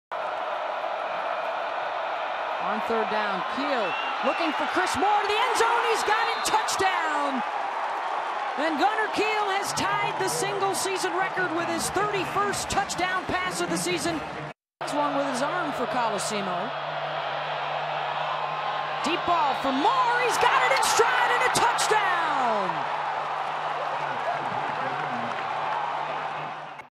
2014 Military Bowl - Cincinnati's Touchdowns
Cincinnati's Gunner Kiel and Michael Colosimo tossed touchdown passes in the Military Bowl presented by Northrup Grumman. The Bearcats fell 33-17 to Virginia Tech (Highlights courtesy ESPN).